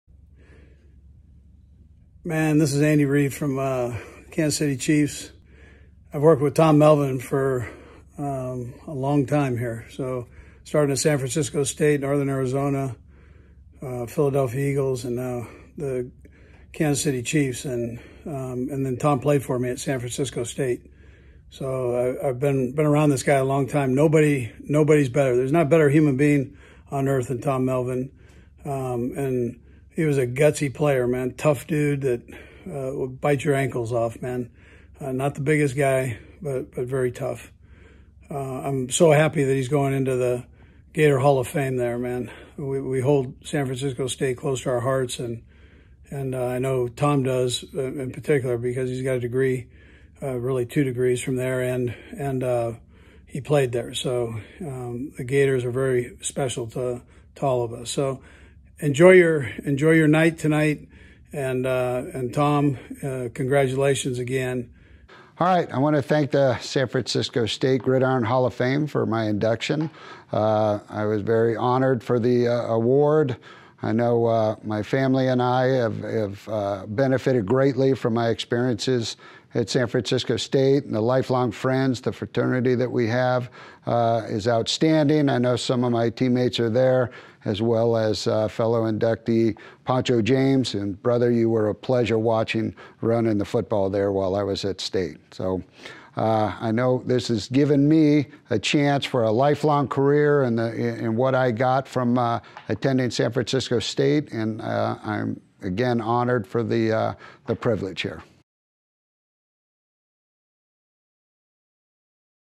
2021 SF State Gridiron Hall of Fame: Tom Melvin
Tom Melvin was inducted into the 2021 San Francisco State Gridiron Hall of Fame.